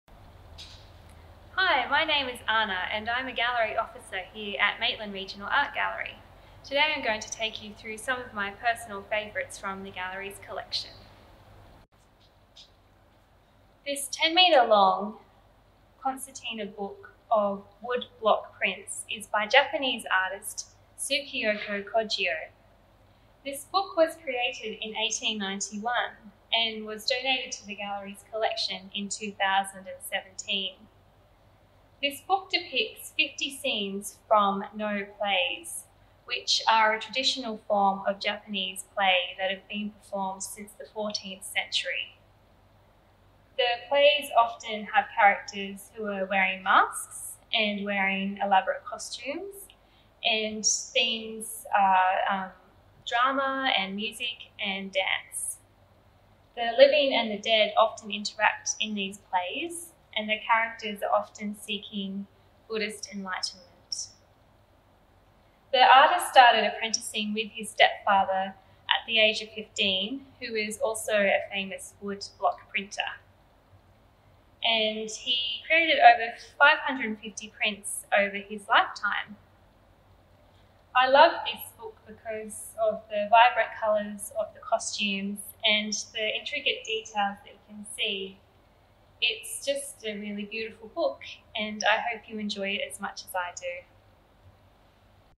MRAG Collection peek | Part 1: Encyclopedia of Noh plays
Join Maitland Regional Art Gallery Officer Anna Buxton Soldal as she explores some of her favourite artworks in the City's Collection in this three part series.

Part 1 looks at a 10m long concertina book of woodblock prints created by Tsukioka Kōgyo. This 'Encyclopedia of Noh Plays' created in 1891, was donated to the Gallery in 2017 by a private donor.

Artwork credit: Tsukioka Kōgyo, Encyclopedia of Noh Plays, 1891, book of woodblock prints on paper, 23.9 x 1080cm (open), Donated under the Australian Government's Cultural Gifts program by Anthony Renshaw in 2017